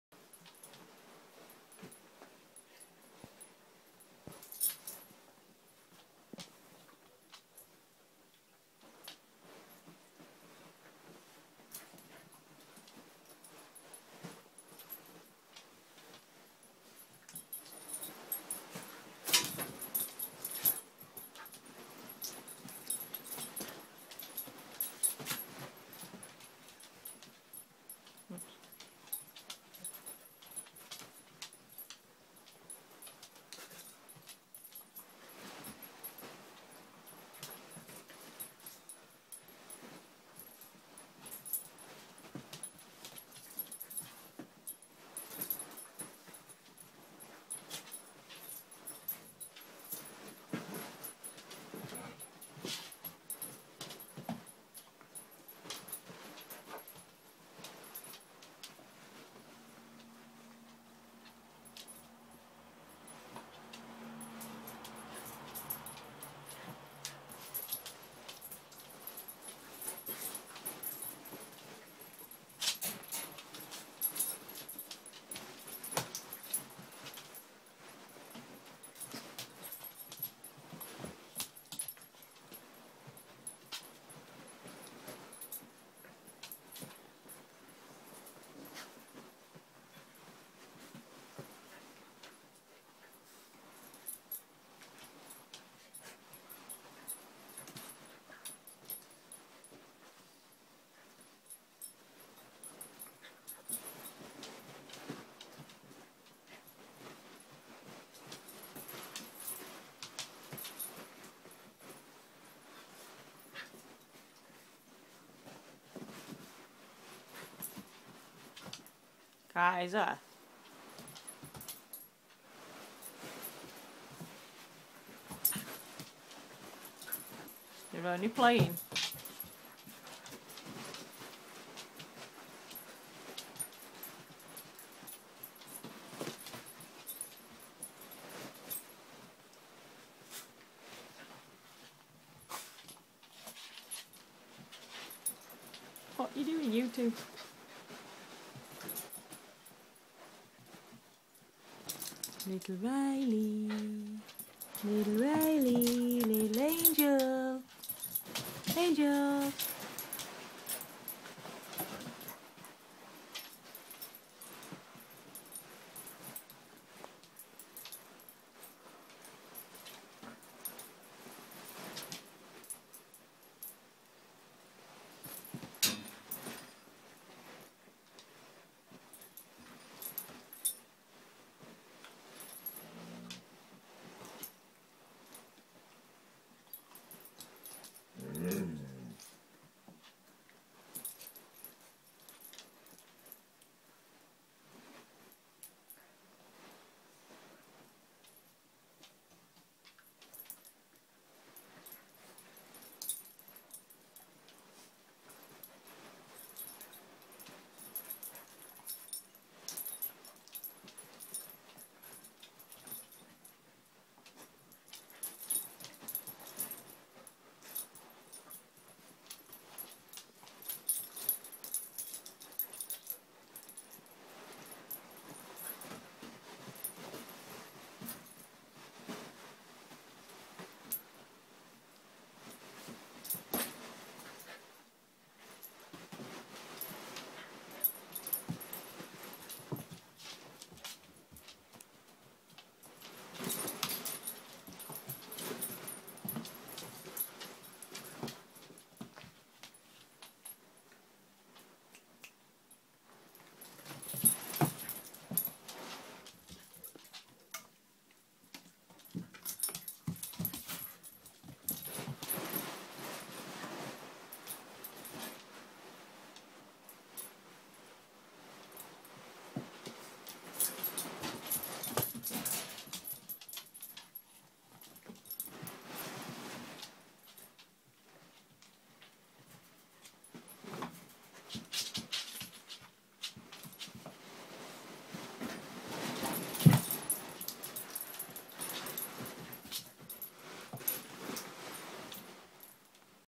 Husky pups playing
My little Angel(5months) & Riley(3months) husky puppies play fighting bless!